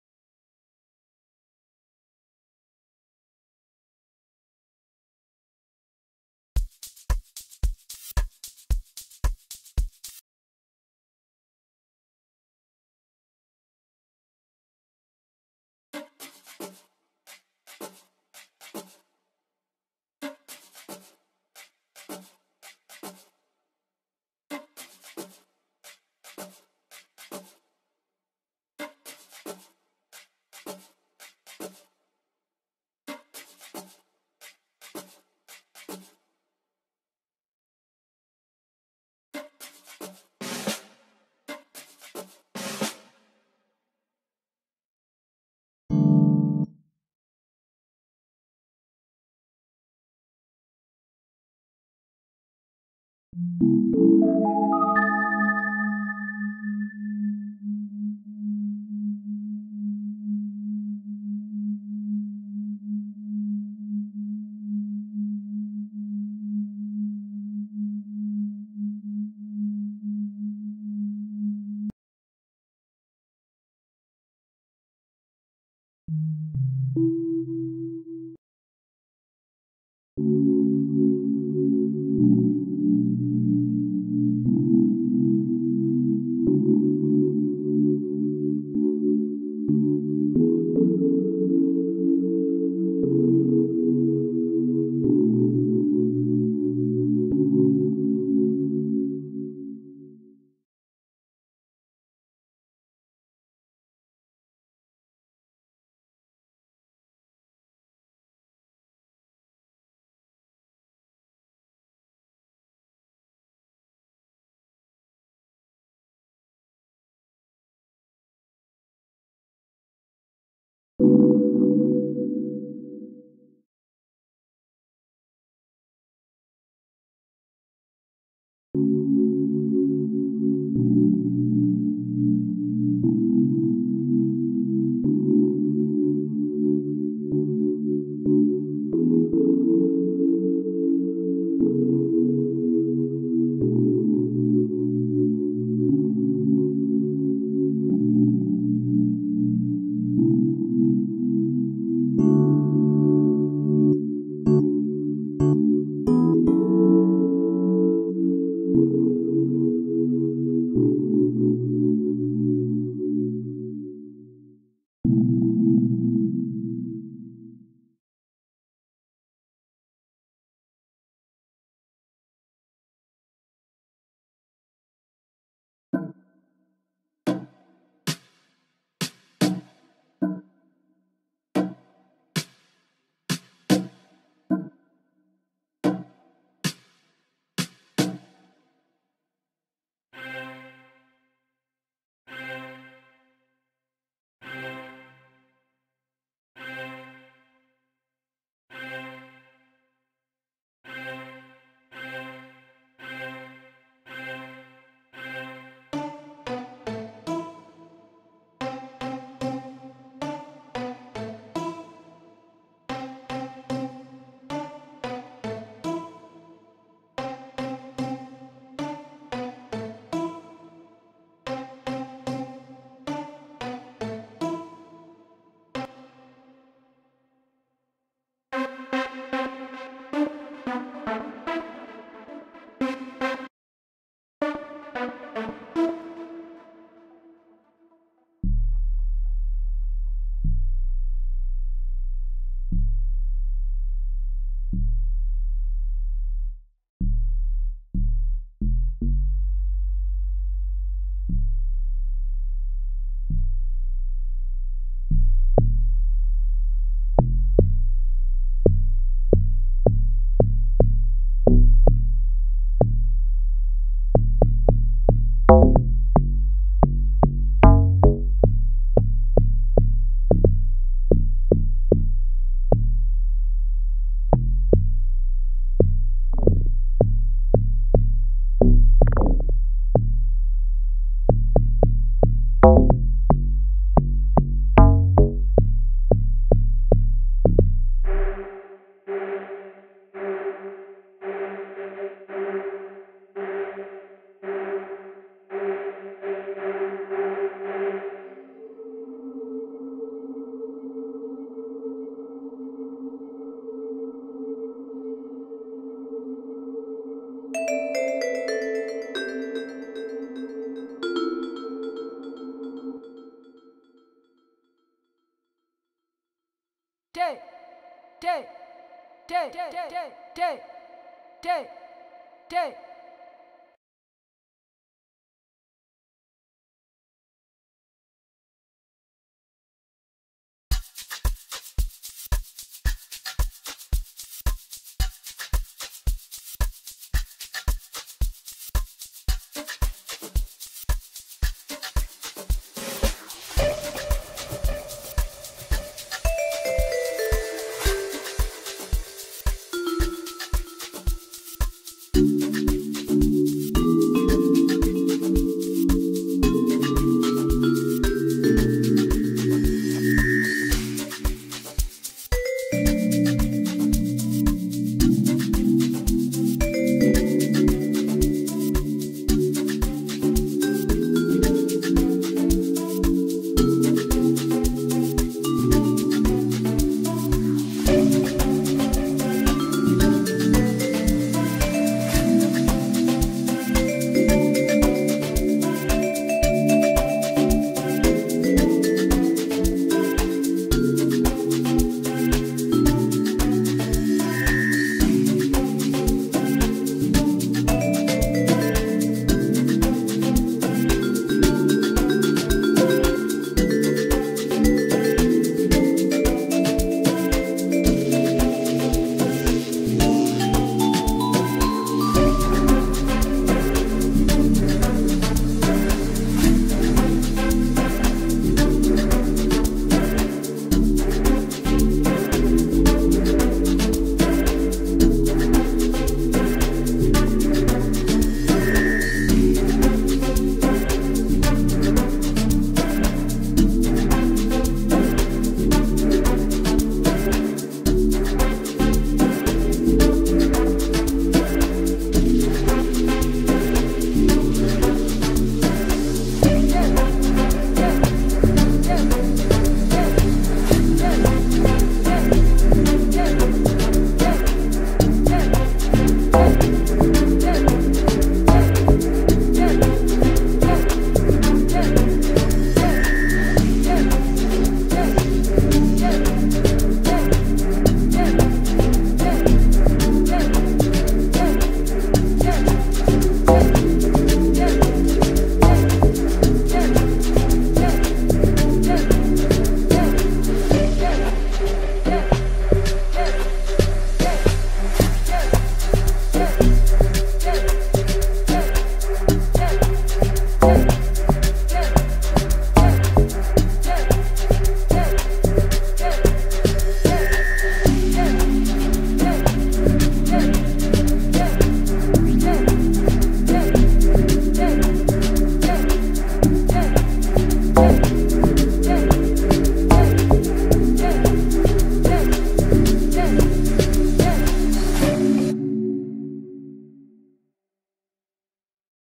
How To Produce Amapiano Like Kelvin Momo, Mick Man & Gaba Cannal Part.025)
•In this video I will be showing how to make a Private Piano Type Amapiano Like Kelvin Momo, Mick Man & Gaba Cannal In Fl Studio 20.

↪Kelvin Momo, Mick Man & Gaba Cannal Tutorial - FL Studio🥭 Private Piano Edition

↪Thank You For Your Support🔥 Danko!!! ROAD TO 1K sub ❤❤❤

by Exquisite MusiQ (Dj Cool 708)